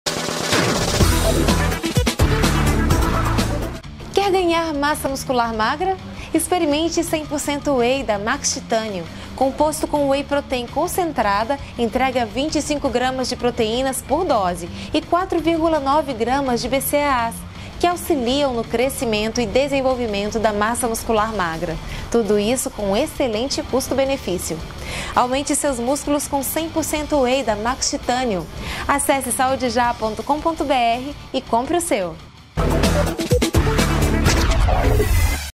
100% Whey - Max Titanium
Quer ganhar massa muscular magra? Experimente 100% Whey da Max Titanium. Composto com Whey Protein Concentrada, entrega 25g de proteínas por dose e 4,9g de BCAAs, que auxiliam no crescimento e desenvolvimento da massa muscular magra, tudo isso, com excelente custo benefício. Aumente seus músculos com 100% Whey da Max Titanium! Acesse e compre o seu!